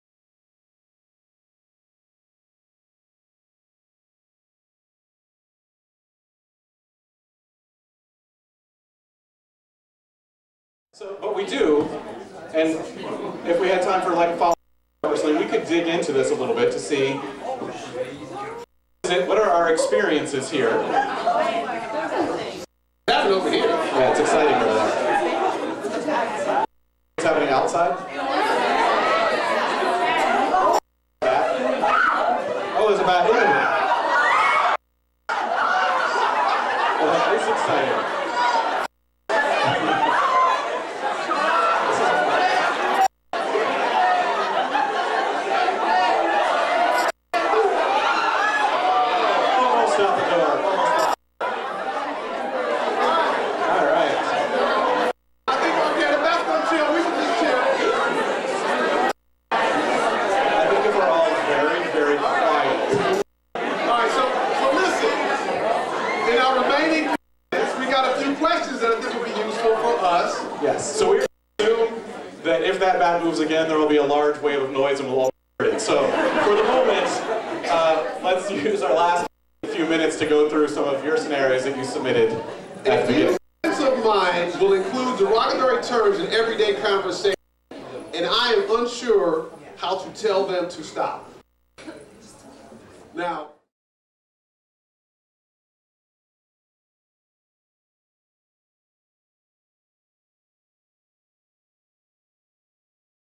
Winged guest at David Campt's lecture on Diversity and Dialogue
A bat grabbed a lot of attention when Dr. David Campt and Matthew Freeman were giving a joint lecture on racial diversity at Elkins and Davis College.